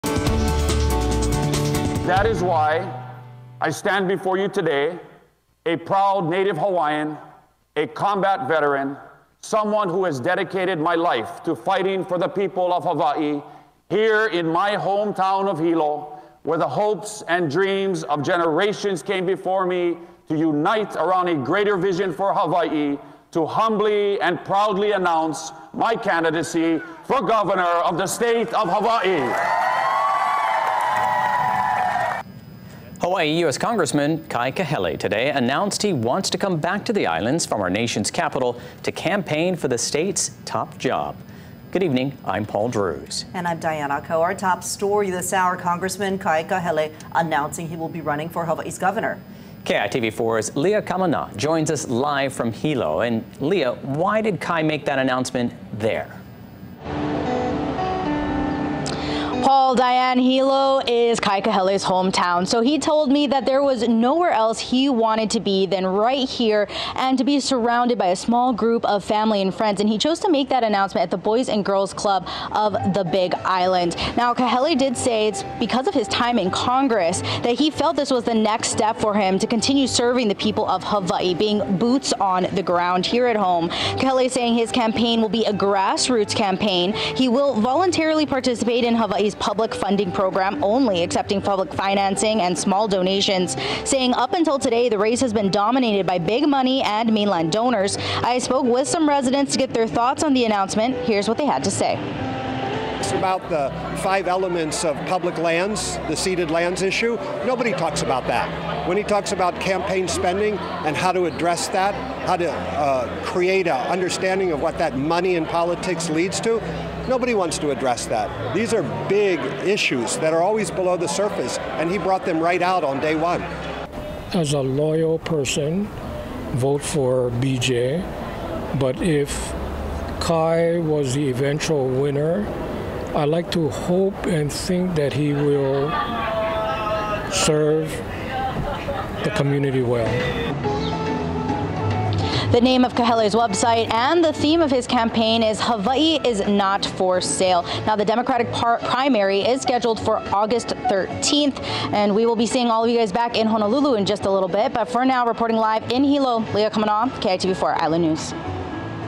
Kahele announces run for governor
The US Congressman shares his reason behind this political decision.